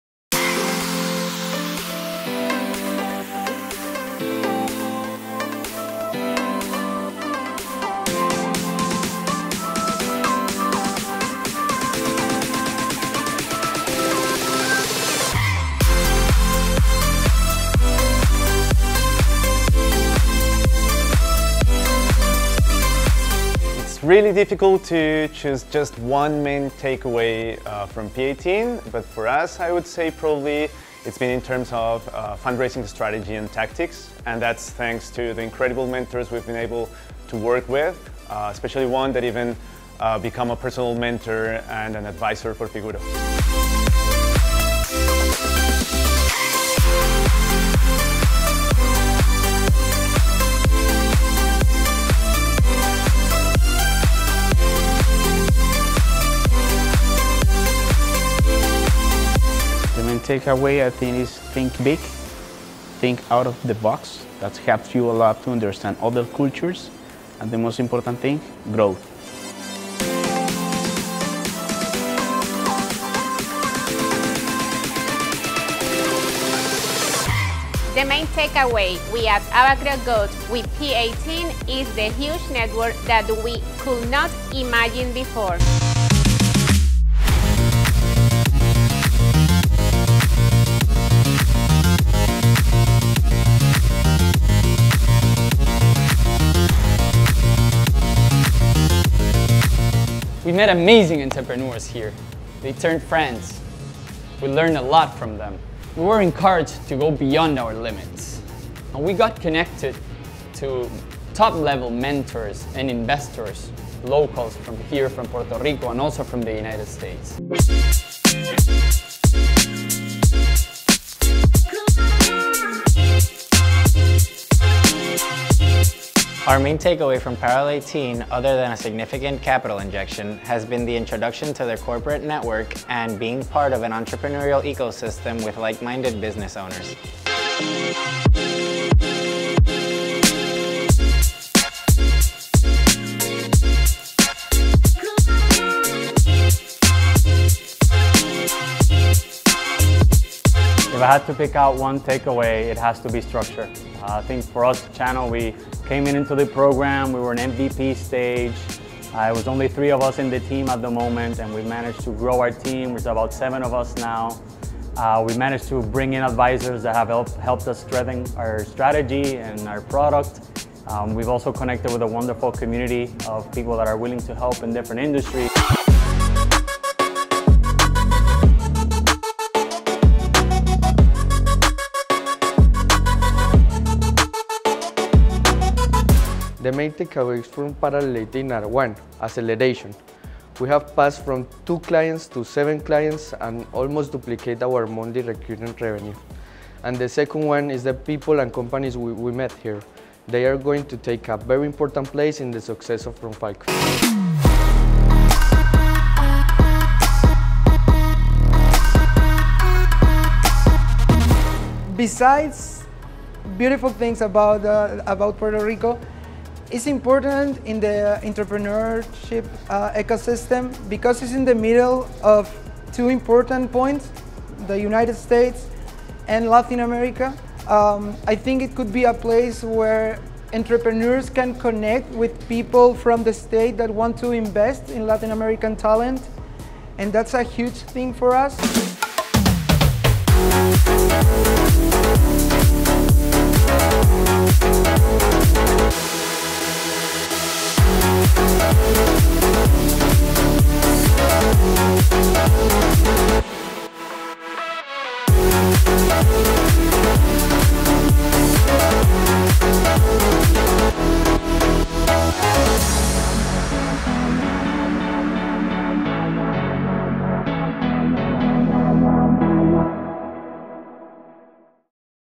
Video Intro Demo Day GenX P18 2022
Ante un público compuesto por inversionistas, socios corporativos y miembros del ecosistema empresarial local e internacional, la Generación X del programa de aceleración internacional P18 de parallel18 presentaron sus soluciones, logros y planes futuros en su tan esperado DemoDay.

Figuro, eSponsor, RunFalcon, RedAtlas, Channel, Abacrop, Lank y Wihom, provenientes de Colombia, Chile y Puerto Rico fueron las empresas participantes. El cohorte 100% latino proviene de industrias como: agtech, fintech, bienes raíces/proptech, insuretech, entre otras.

Youtube: Puerto Rico Science, Technology, and Research Trust